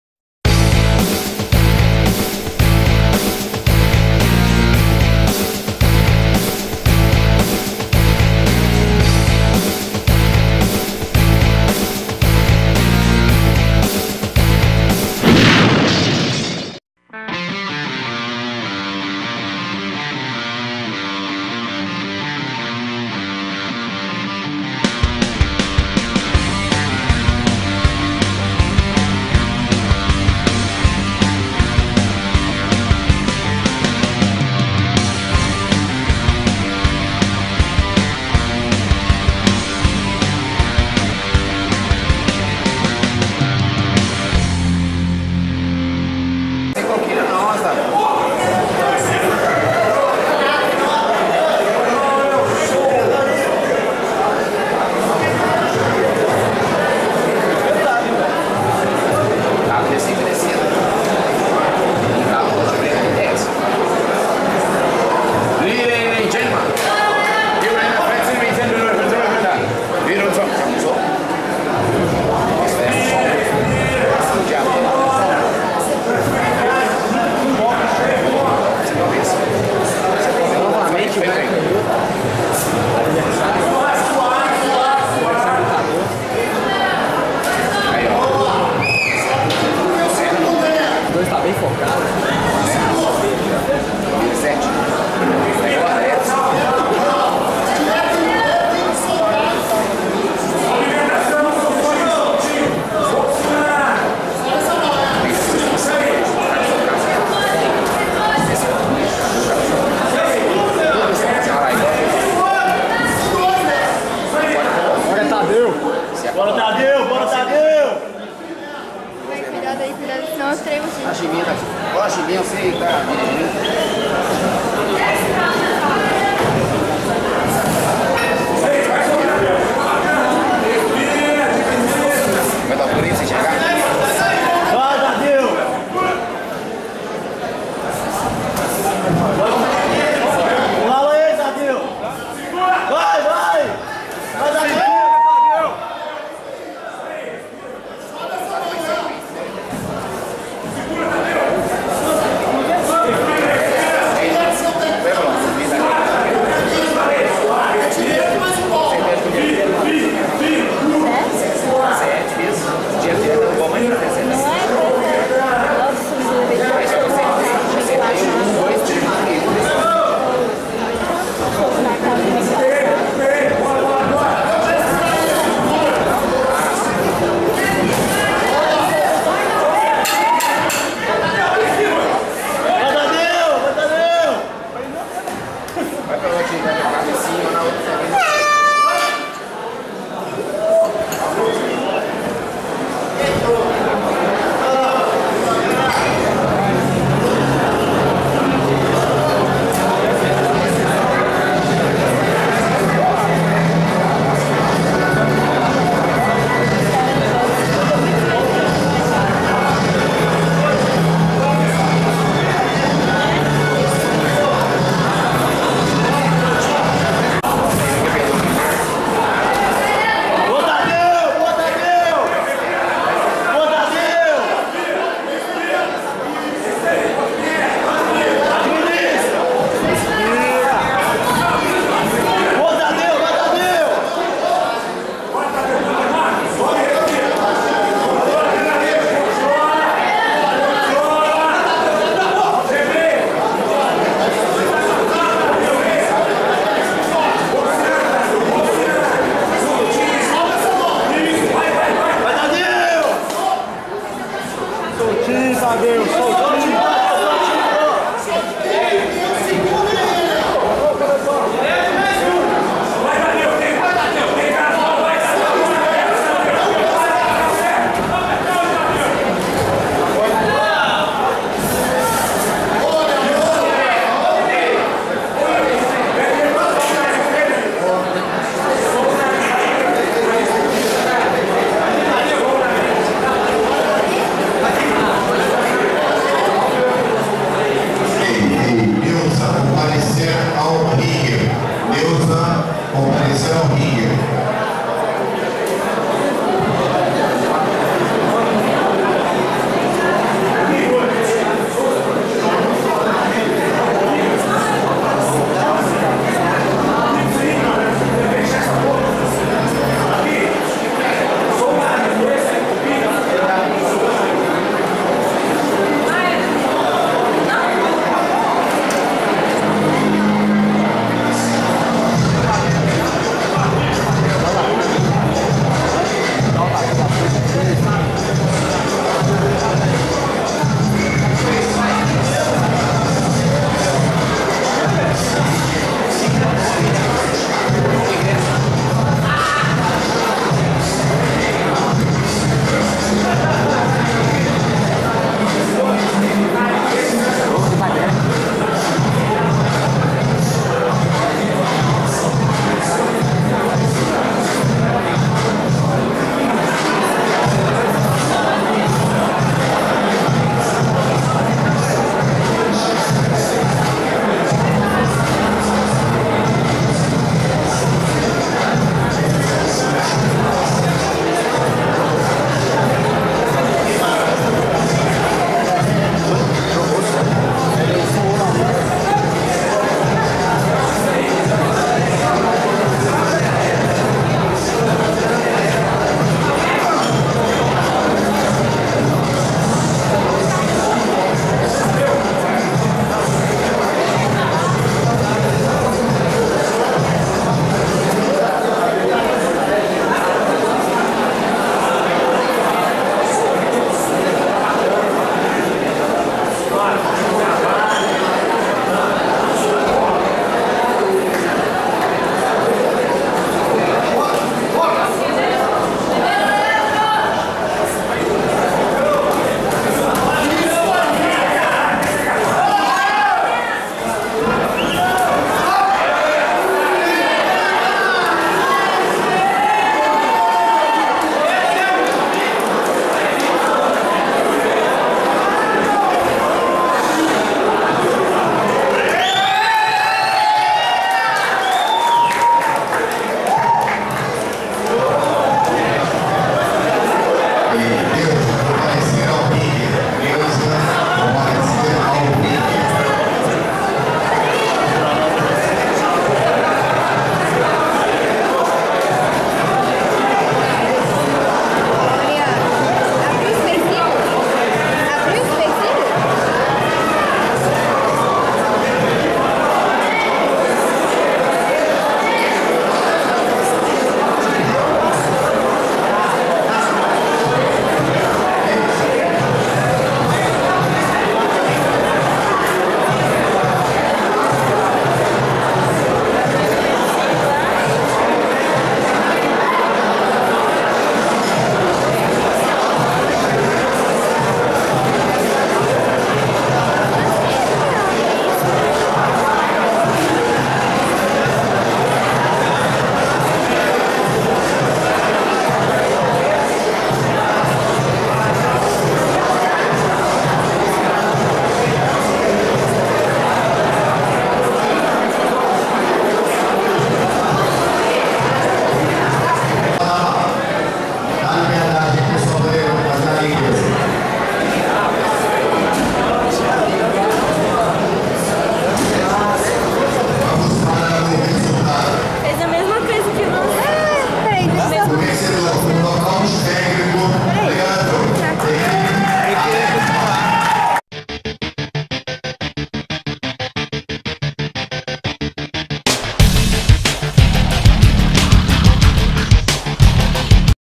Leandro vs Thadeu - BH Sparta 3
Leandro (Alex Rocha Team) venceu no segundo round por nocaute técnico Thadeu Fraga (Dalton Team).